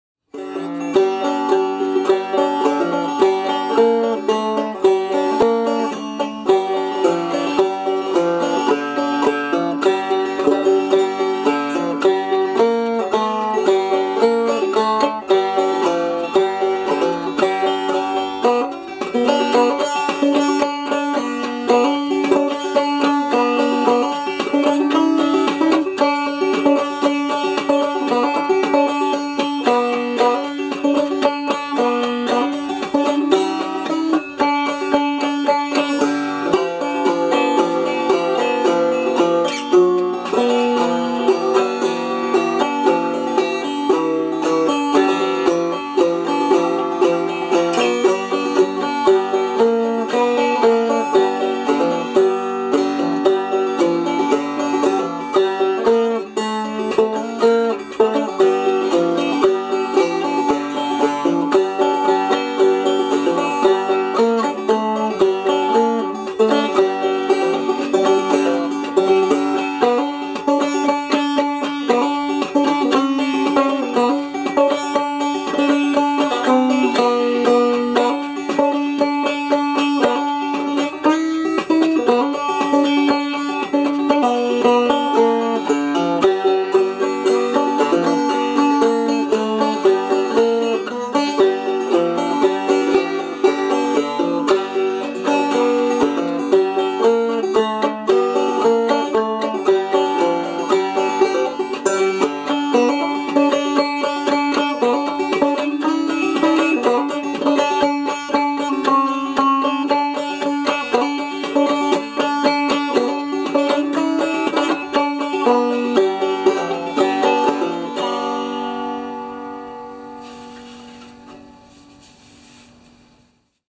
Hubcap Banjo 1
Here’s another real nice looking open back with a gig bag:

This is the 47 Ford hubcap banjo that Tom Ford built for me..